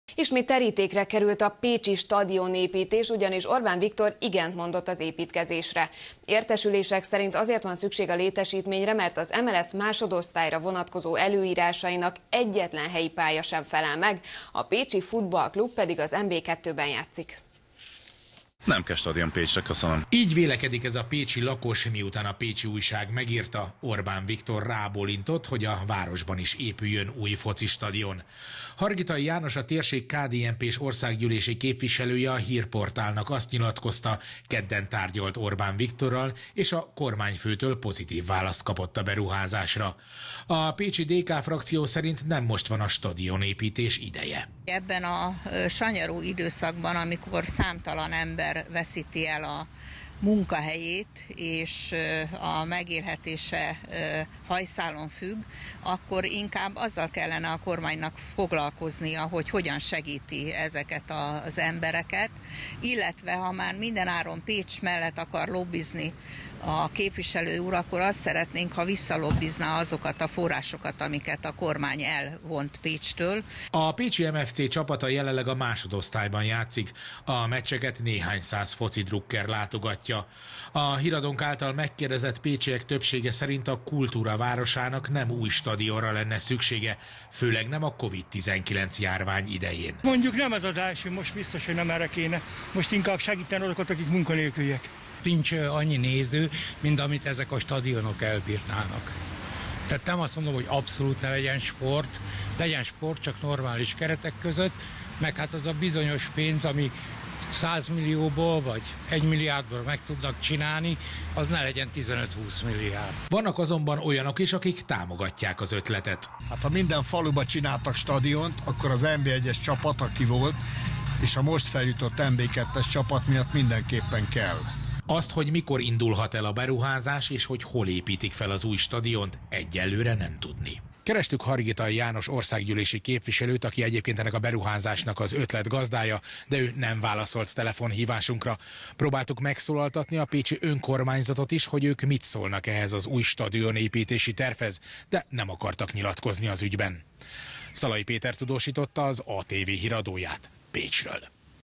Új futballstadion épül Pécsett
Ismét terítékre került a pécsi stadionépítés, ugyanis Orbán Viktor igent mondott az építkezésre. Értesülések szerint azért van szükség a létesítményre, mert az MLSZ másodosztályra vonatkozó előírásainak egyetlen helyi pálya sem felel meg.